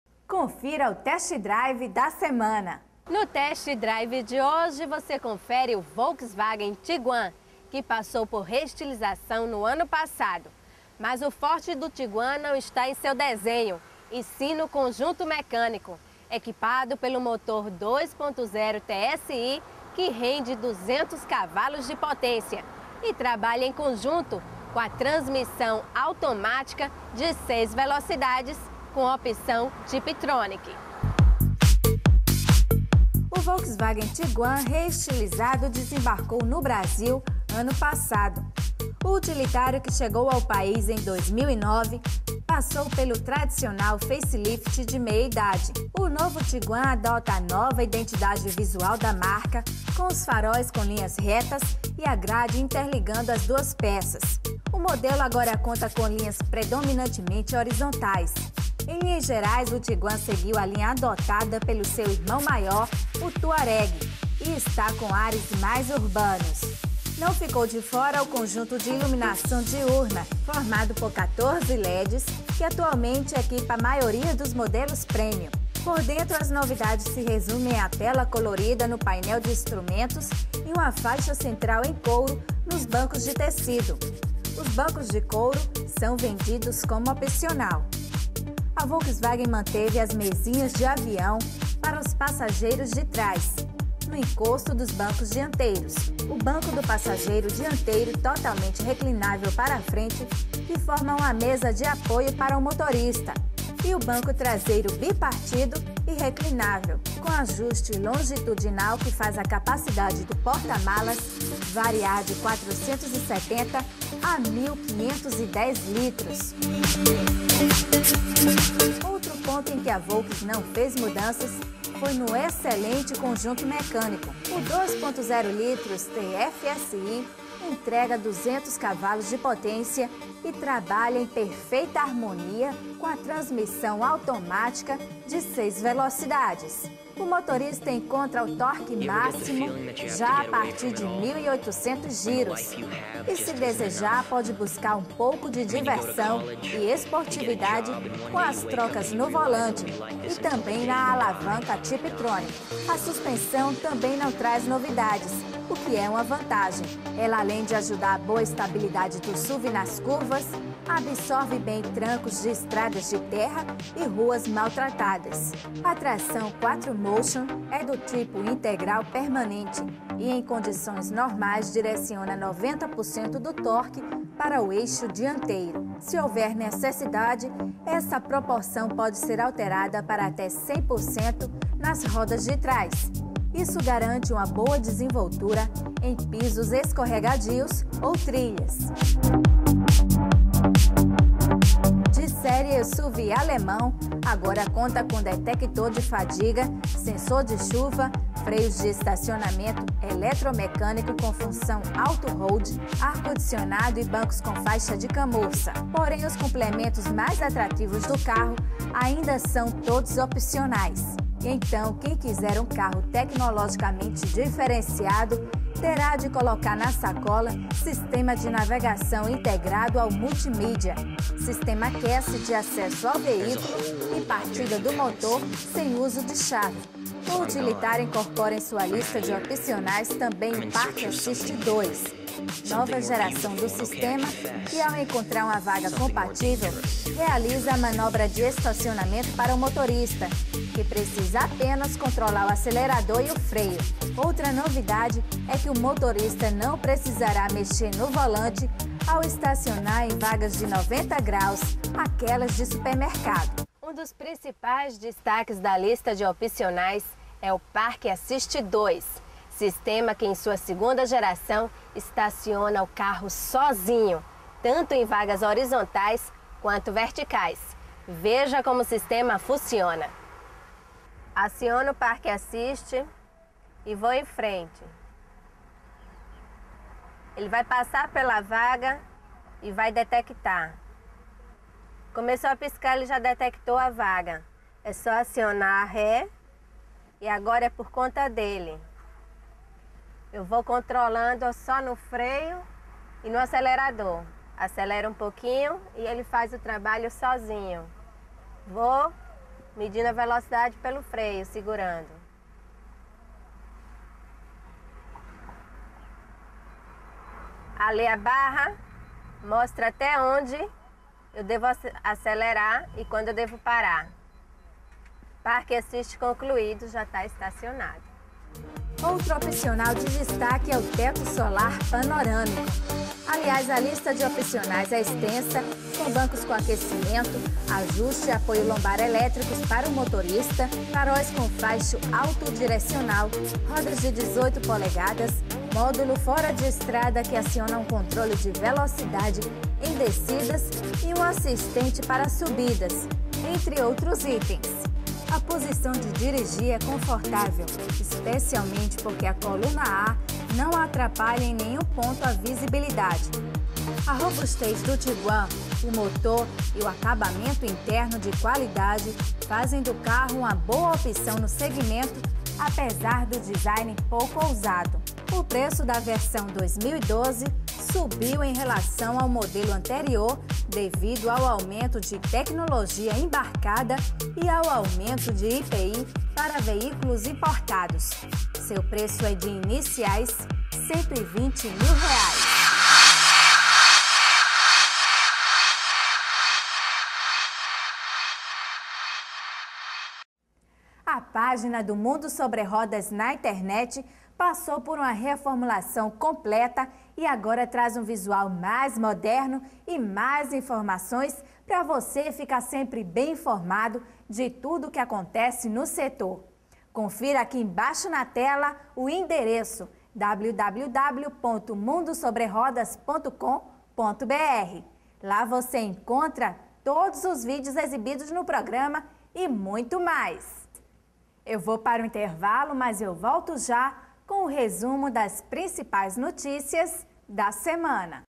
Test drive Volkswagen Tiguan 2012
Todos os detalhes do SUV compacto premiun e mais demonstração do Park Assist II